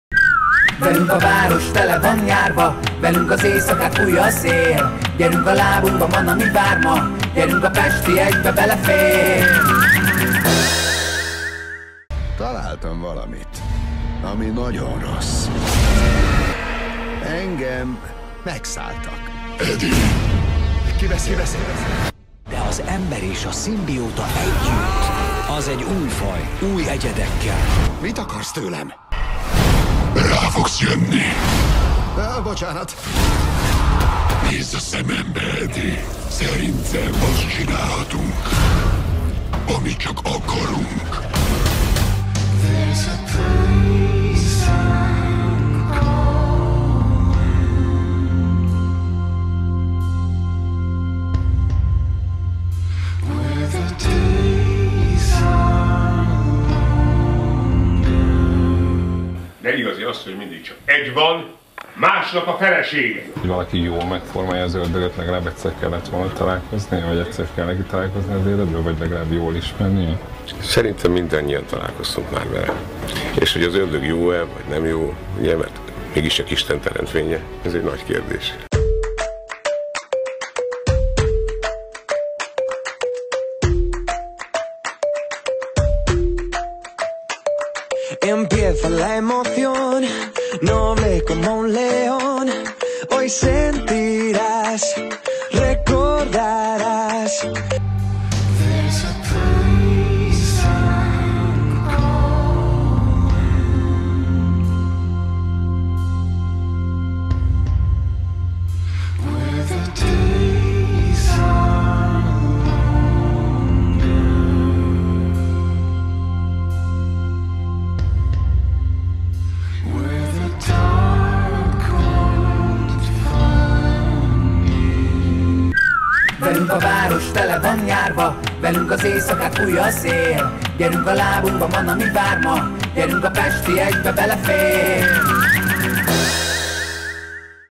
Heti ajánló október 1-7.
Ez a hét embertelen. Idegen lények, állatok és maga az ördög. Az izgalmakat le kell vezetni egy Pesti Est-koncerttel :)